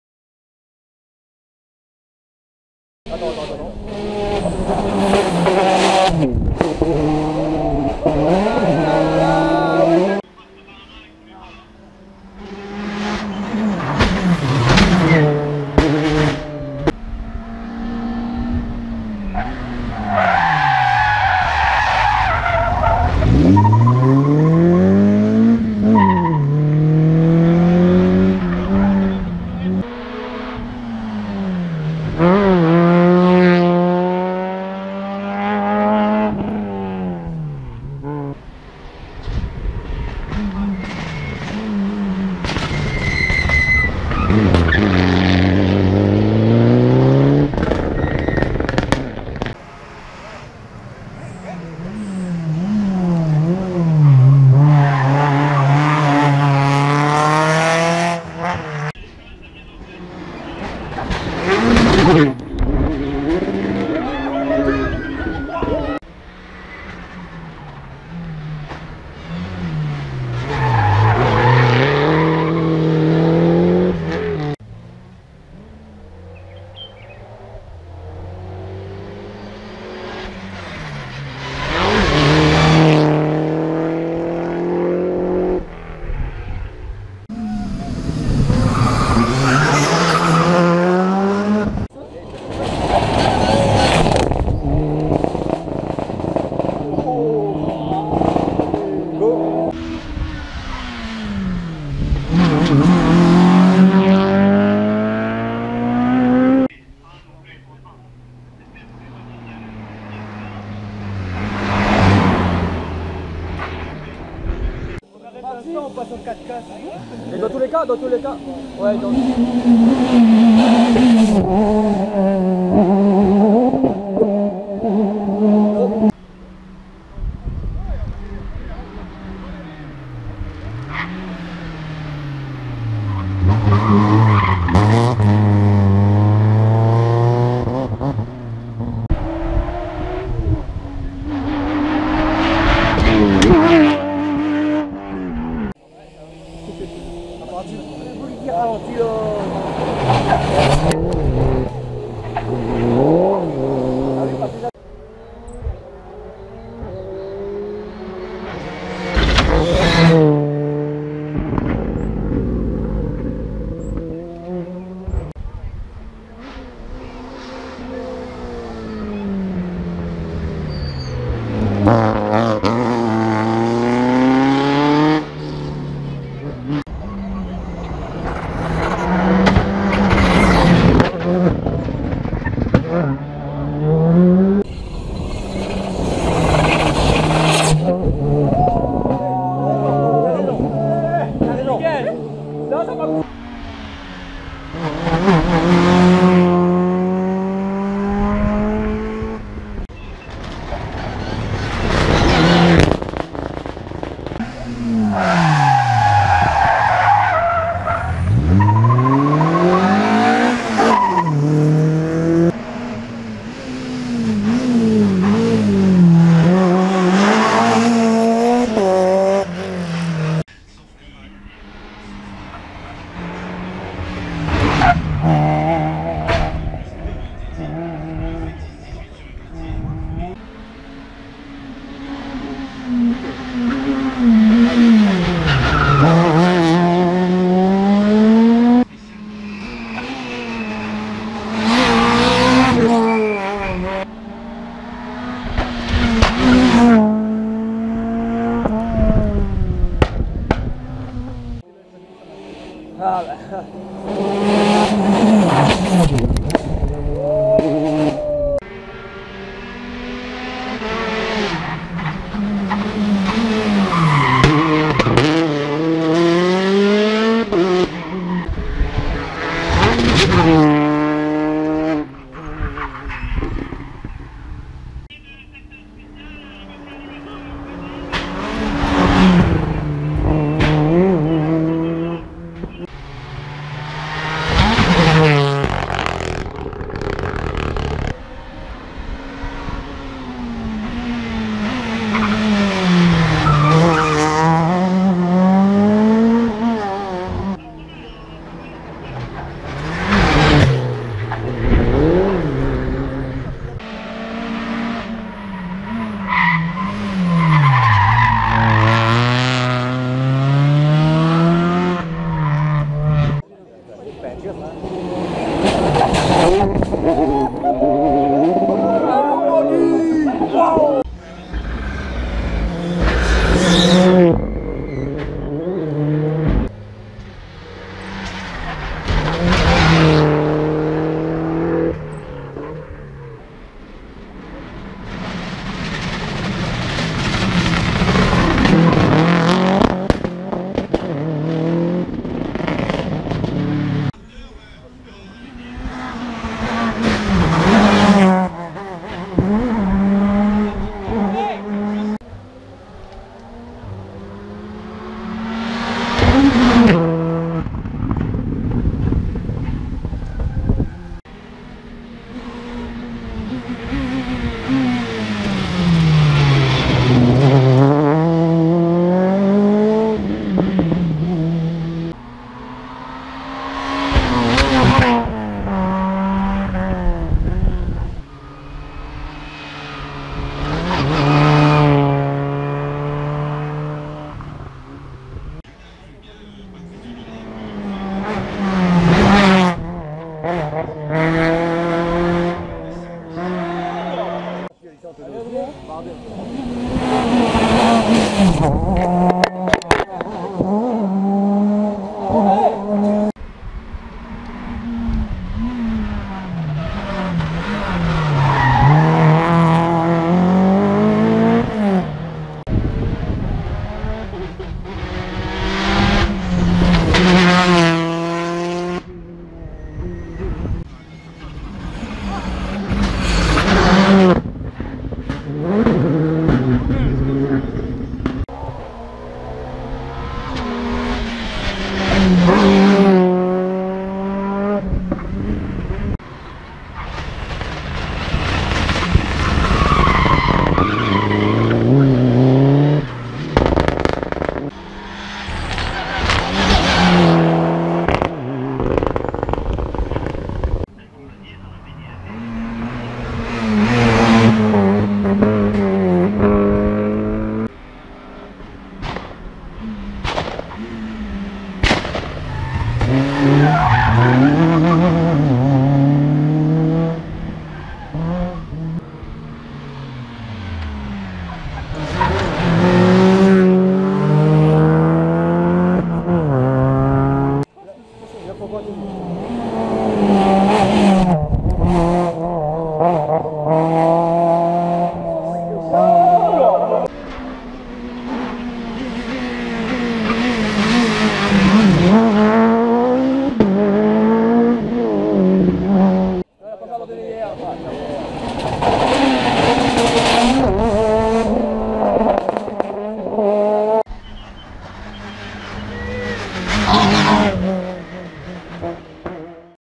Rallye De bourbon 2018
Glisse Tout droit passage de oufff.

Des Vidéos dans différent passage !!

Bye Pixel Rallye Réunion